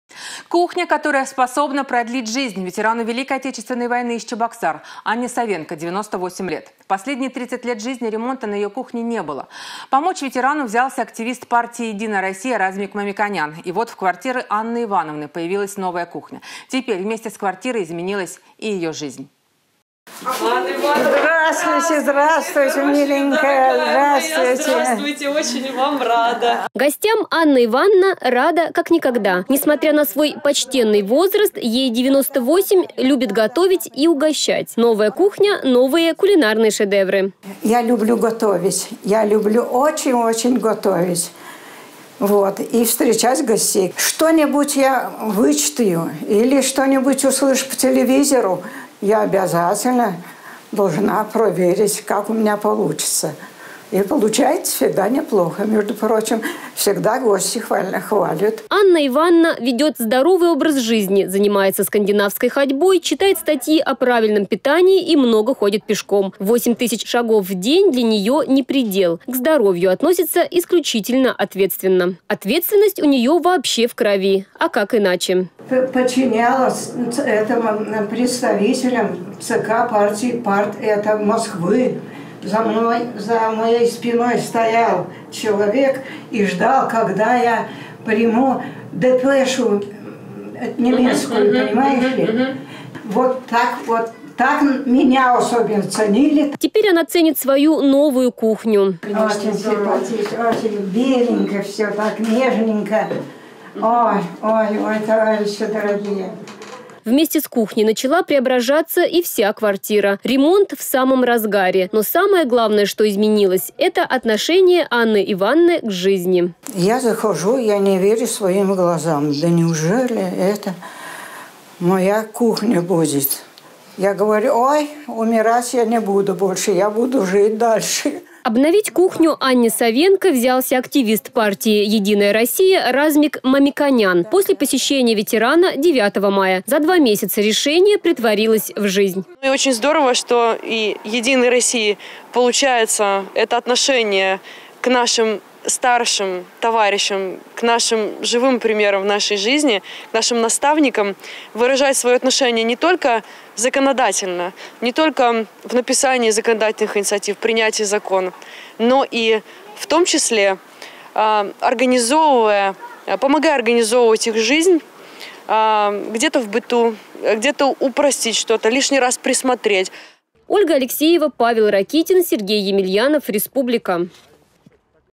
При поддержке «Единой России» ветерану ВОВ сделали ремонт на кухне
Кухня, которая способна продлить жизнь. Ветерану Великой Отечественной войны из Чебоксар - Анне Савенко 98 лет. Последние 30 лет жизни ремонта на ее кухне не было. Помочь ветерану взялся активист партии «Единая Россия» Размик Мамиконян. И вот в квартире Анны Ивановны появилась новая кухня. Теперь вместе с квартирой изменилась и ее жизнь.